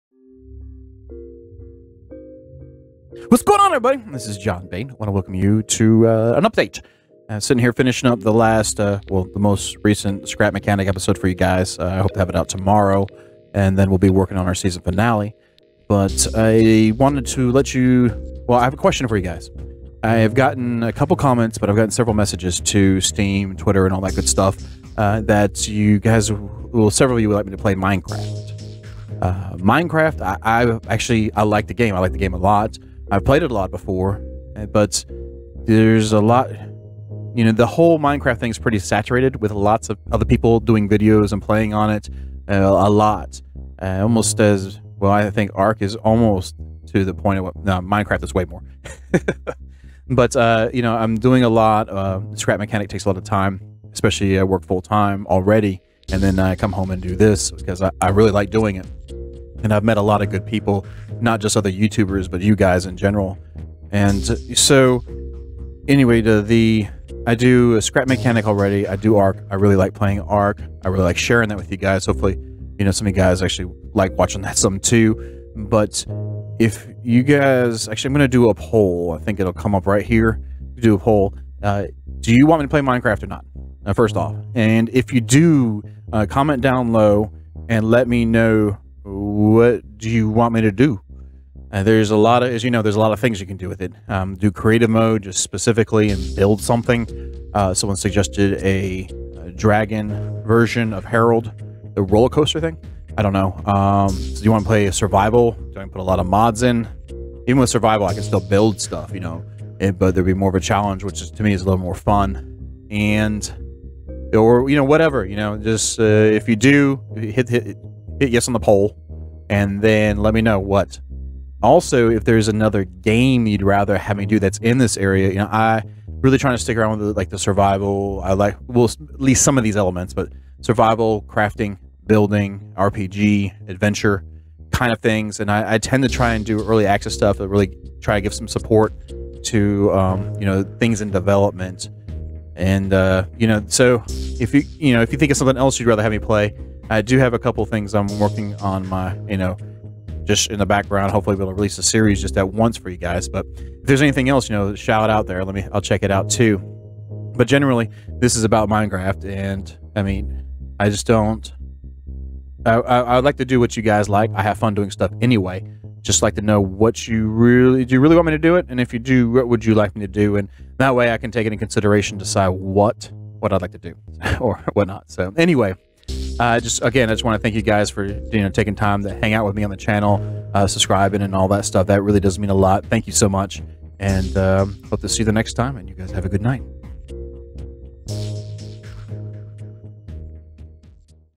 Do you want me to play Minecraft??
So,

I've gotten requests in the comments and messages to play minecraft. How many of you want me to play it? If so, what do you want me to do? Click the poll and tell me what below!

j0hnbane

BE A PART TO THIS!